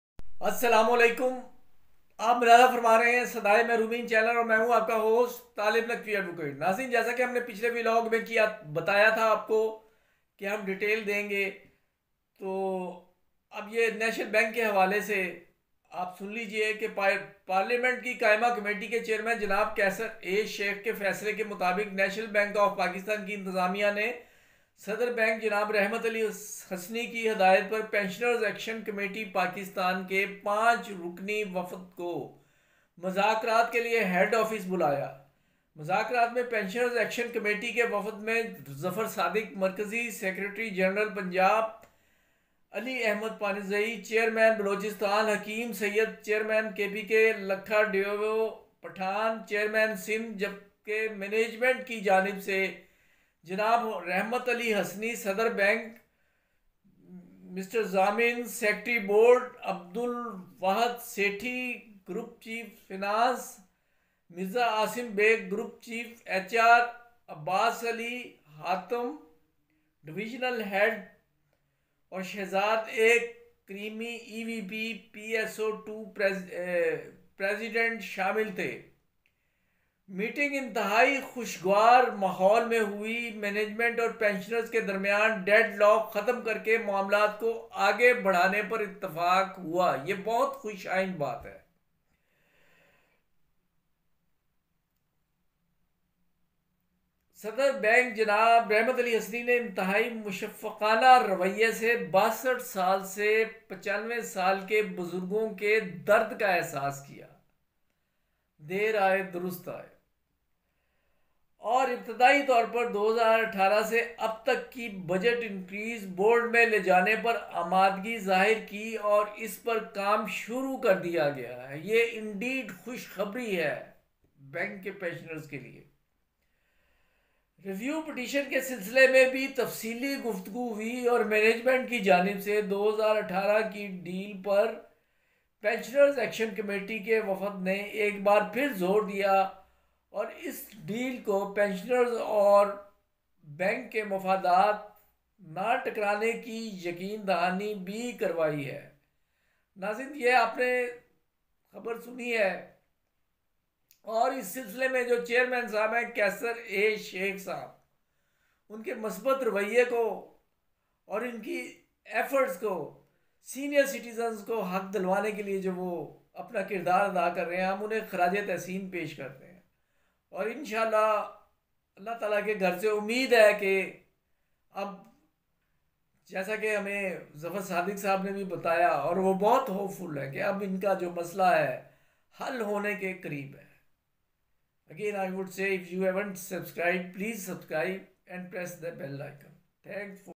EOBI PTCL AND NATIONAL BANK NEWS MEETING WITH CHAIRMAN STANDING COMMITTEE AND ALL STAKE HOLDERS
@talibnaqviadv  We like Eobi latest news have been giving bank news as bank plays an important role in stability of a state's economy and among banks after State bank National bank has its own character Export import remittances inflation loans all things are controlled mainly by National bank but its current and former working force facing poverty in this age of inflation EOBI PTCL AND NATIONAL BANK NEWS MEETING WITH CHAIRMAN STANDING  COMMITTEE AND ALL STAKE HOLDERS